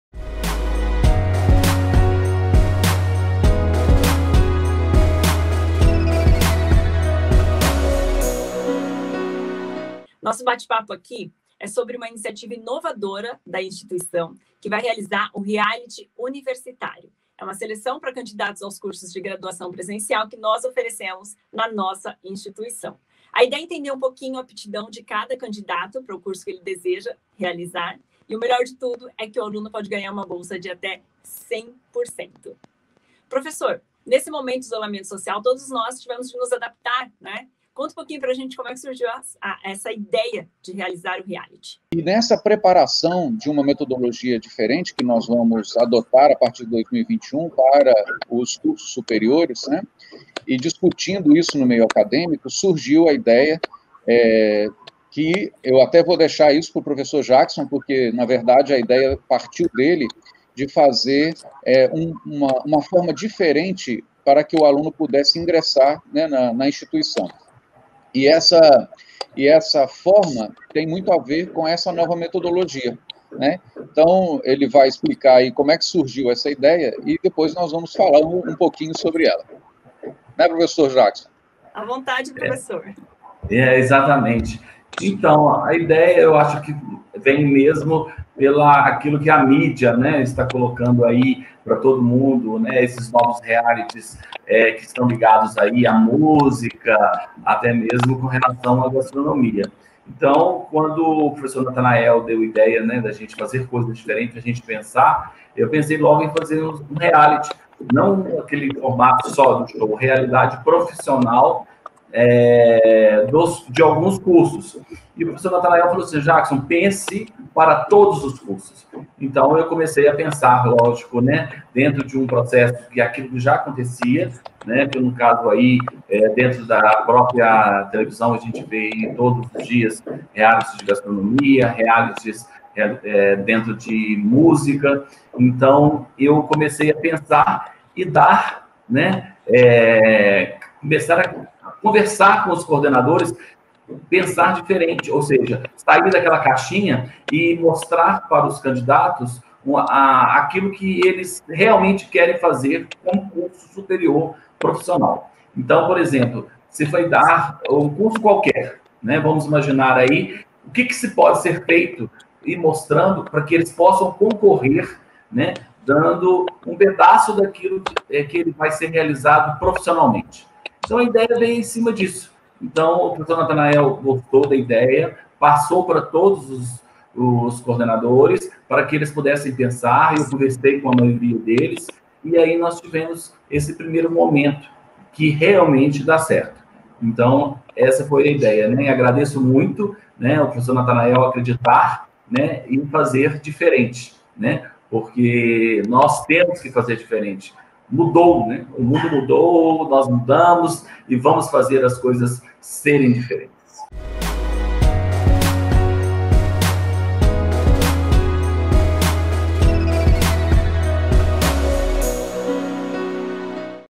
#01 - Como surgiu a ideia do Reality Universitário?
Conheça o nosso Reality Universitário Mentes do Futuro. Na entrevista, o reitor do UniSant’Anna, professor Natanael Atilas Aleva, e o coordenador do curso de Gastronomia, Jackson Cruz Cabral, contam tudo sobre a iniciativa inovadora da instituição que tem agora mais uma forma de seleção para candidatos aos cursos de graduação presencial . Vem entender como o nosso reality avalia a aptidão dos alunos em cada área de interesse. Além de ser uma forma inovadora de vestibular, pode garantir uma bolsa de até 100%.